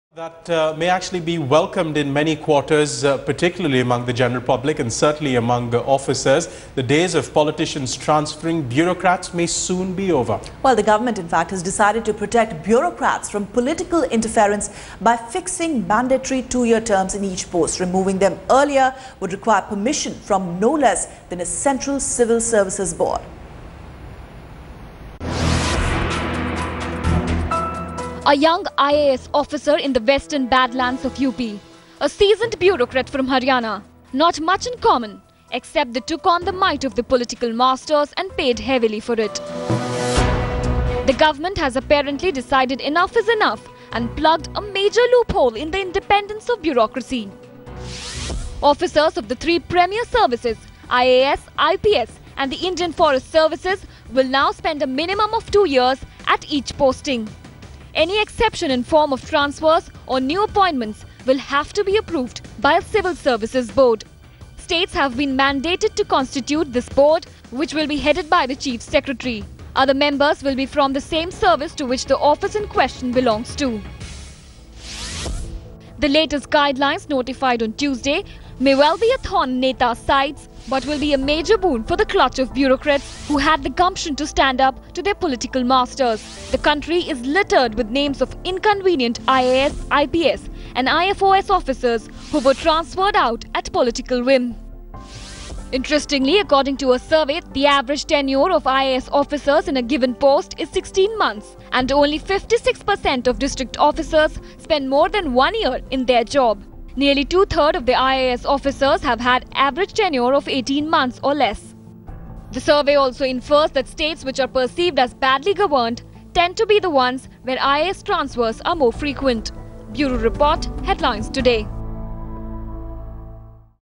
Freedom from political transfers for Babus?
If there is one section of government officials that pays the highest price for honesty, it is the bureaucrats i.e the IPS, IAS and the IFS officers. In a bid to protect the bureaucrats from unlawful transfers, the government is all set to fix the minimun tenure of a bureaucrat to two years. A transfer before the completion of this predetermined tenure would require the permission of the Central Civil Services board.